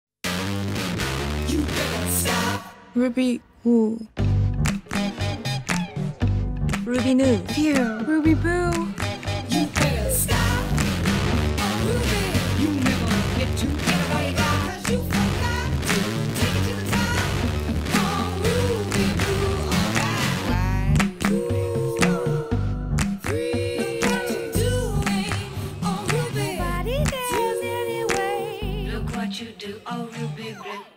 M·A·C Ruby's Crew
Iconico. Intenso. (Br)Amato da tutti.💋⚡

Nella storia dei rossetti rossi, "Ruby Woo" merita di certo un posto d'onore!👑
Ora, è addirittura disponibile in 4 tra i nostri più amati finish:

💄Retro Matte Lipstick "Ruby Woo";
💄Powder Kiss Lipstick "Ruby New";
💄Powder Kiss Liquid Lipcolour "Ruby Boo";
💄Retro Matte Liquid Lipcolour "Ruby Phew!".

Scoprili in tutti i MAC Store monomarca o online su maccosmetics.it, a questo